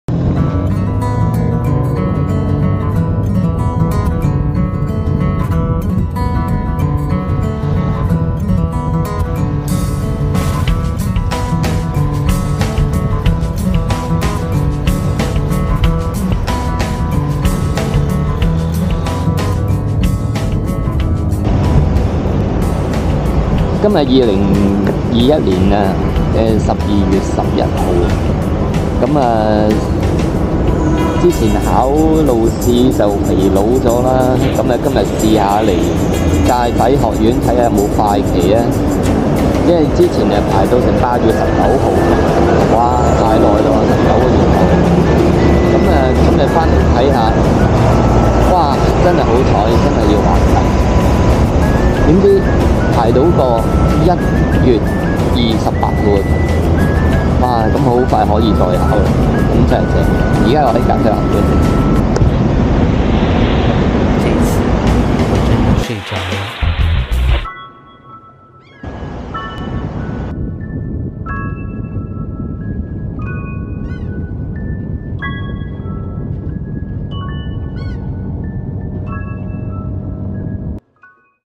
20211211 去沙田駕駛學院問快期
20211211 嘗試去沙田駕駛學院排路試快期